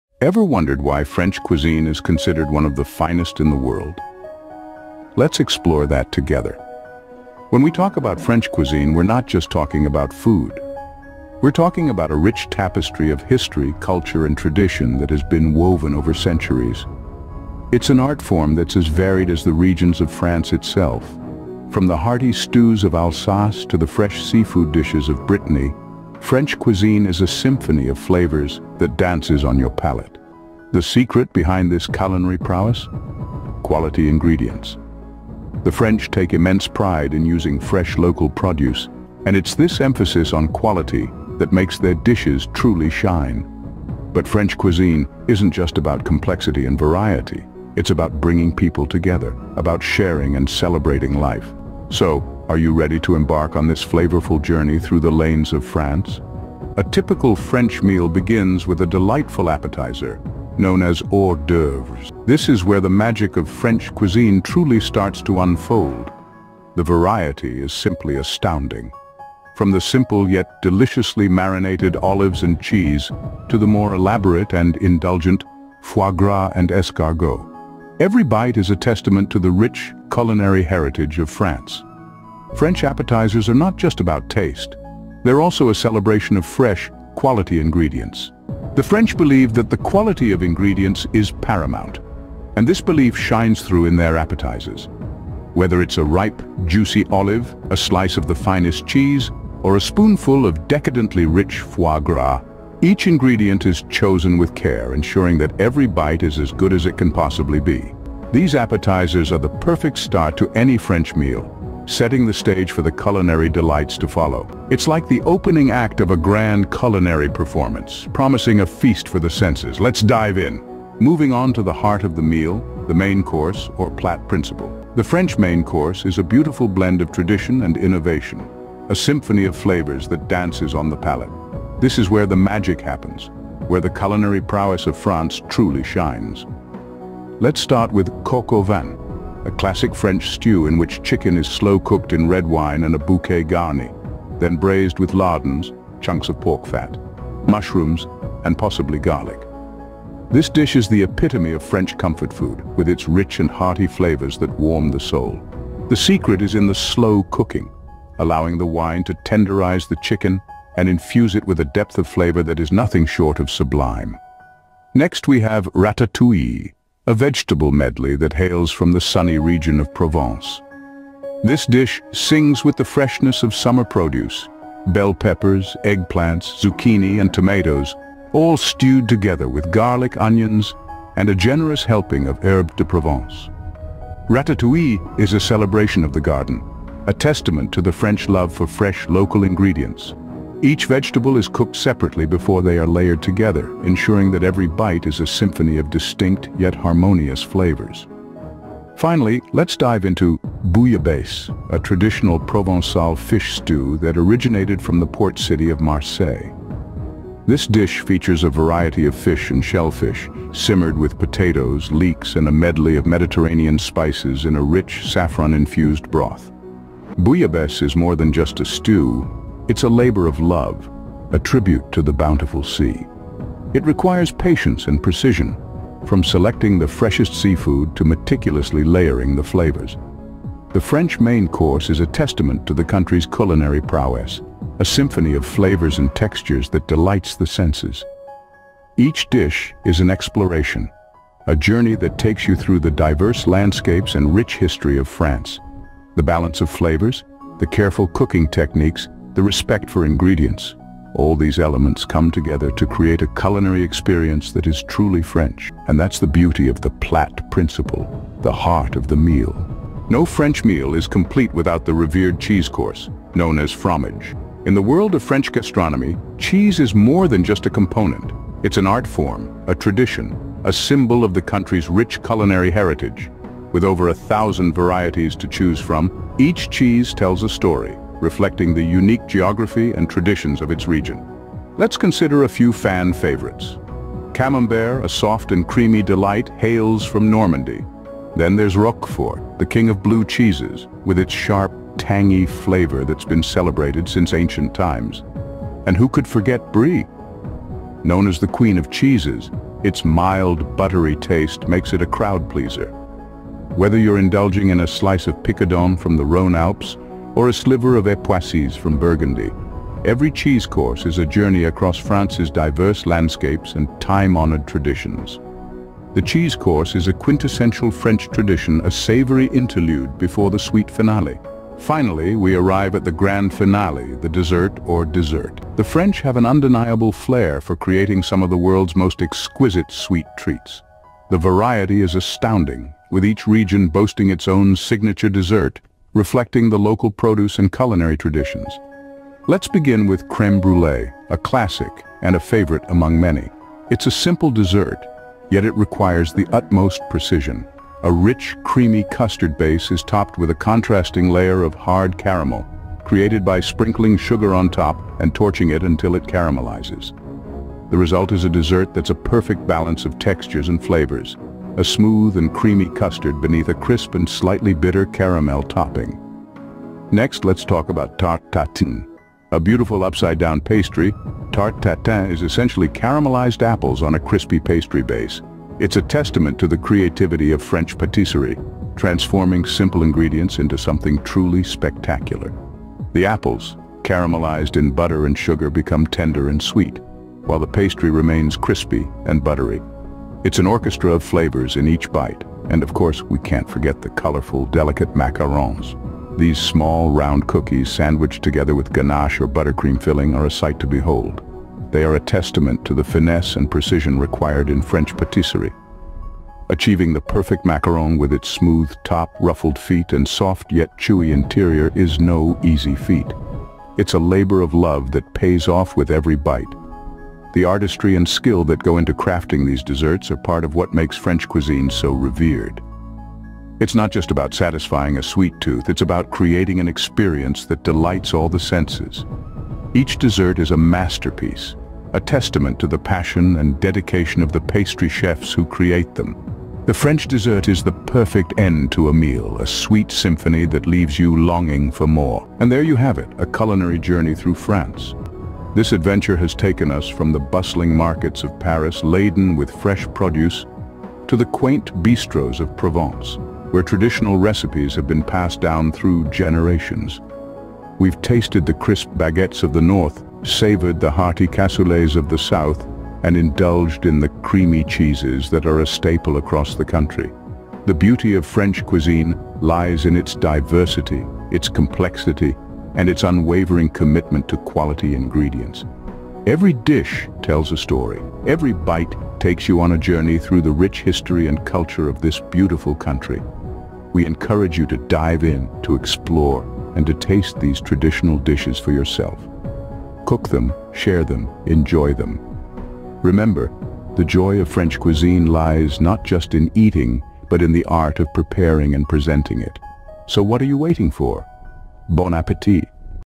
Culinary Journey A Taste of France
Join us on a mouth-watering journey through the rich culinary traditions of France! In this immersive video, we will take you on a vibrant tour of local markets, get hands-on experience cooking with talented chefs, and delve into the fascinating history behind each exquisite dish. Get ready to tantalize your taste buds and learn the secrets of French cuisine like never before! 🇫🇷🍴
See Less

OUTLINE:
00:00:00
Bonjour French Cuisine!
00:00:56
The Appetizing Start!
00:02:02
The Hearty Main Course
00:04:22
Cheese, Glorious Cheese!
00:05:24
The Sweet Finale
00:07:27
Au Revoir!